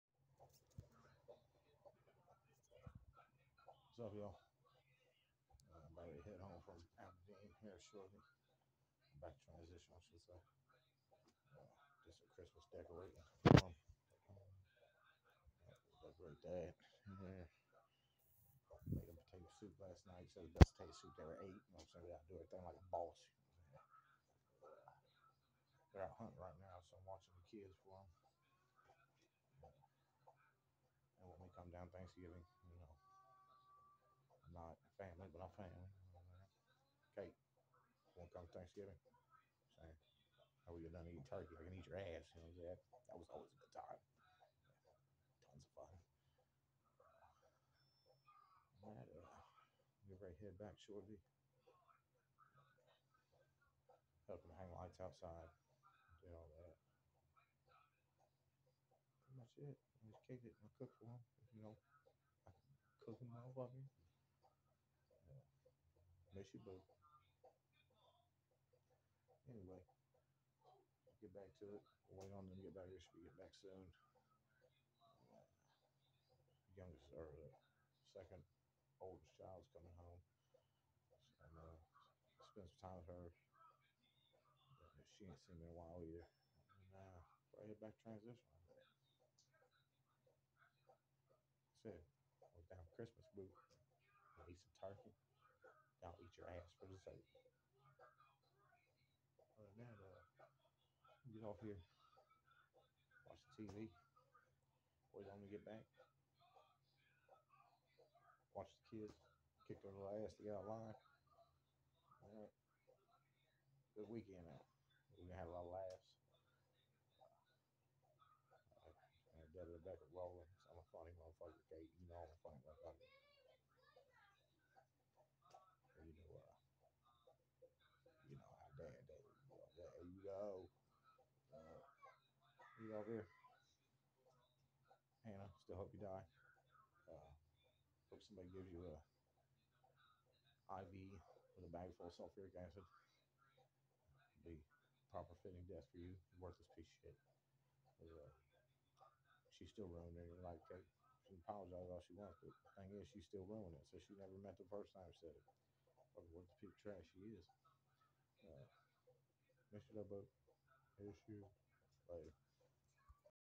getting ready head back to west union
Xmas decoration this weekend Kate you want to come down for Thanksgiving we can eat turkey then I can eat your ass for dessert that was always a good time boo miss ya 🙋🤷😋😉